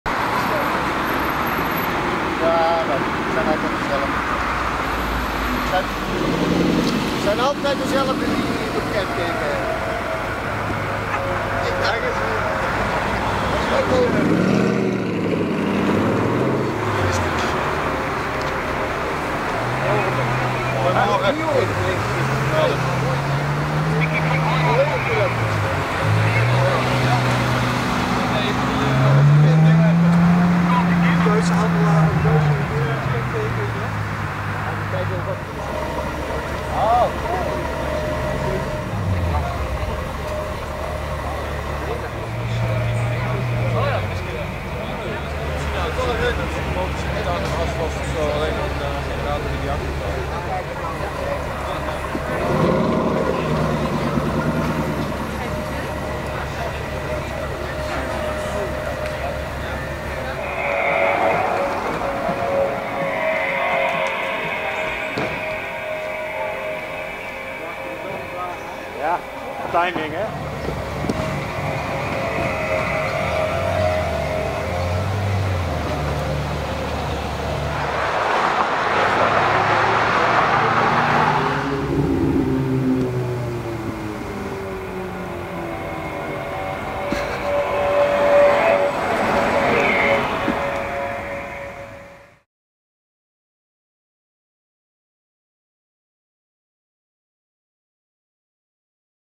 Fisker Karma ES Hybrid - In Action!! - 1080p HD
Hans records the all new 2012 Fisker Karma. The Fisker Karma is an entirely new car, from an entirely new car company. The Fisker Karma is a plug-in hybrid luxury sports sedan. The Fisker Karma features the world's largest seamless solar glass roof for a production vehicle. Despite its eco-friendly background the Karma features a 125 mph (201 km/h) top speed and is capable of reaching 60 mph (97 km/h) from a standstill in 5.8 seconds.

- Hans